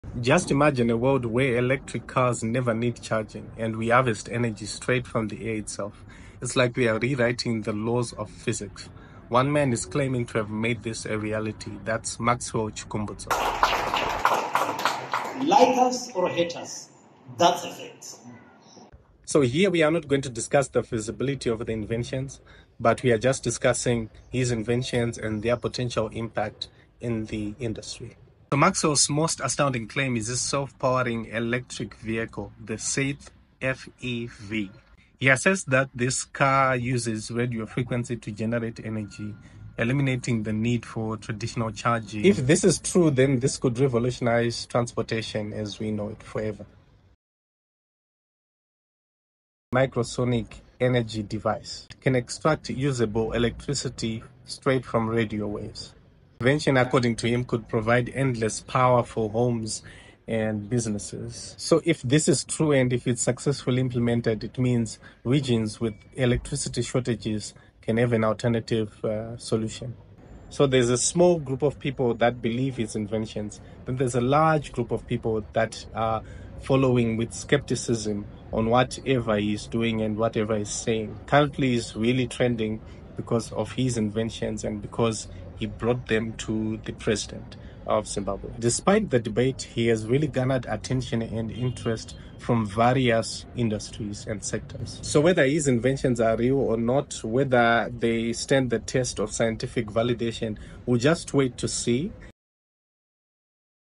Maxwell Chikumbutso Inventions: Self Driving Cars and Free Energy
Maxwell Chikumbutso Zimbabwean Inventor unveils Self Powered Cars and Free Microsonic Energy in Zimbabwe – A Breakthrough or a Hoax?. Imagine a world where electric cars never need charging and can generate their own power while driving. Maxwell Chikumbutso, a self-taught Zimbabwean inventor, claims to have created just that—a self-powered car using microsonic energy and radio frequency technology. But is this truly a scientific breakthrough, or is it just another too-good-to-be-true invention?

In this video, we explore:
✅ The Maxwell Chikumbutso car – A self-powered EV from Africa
✅ How the Saith Technologies Free Energy Car supposedly works
✅ The self-charging electric car and its implications for the EV revolution
✅ Could this be the future of transportation, or just another perpetual motion hoax?

🎥 Watch until the end to uncover the truth about Maxwell Chikumbutso’s inventions and decide for yourself!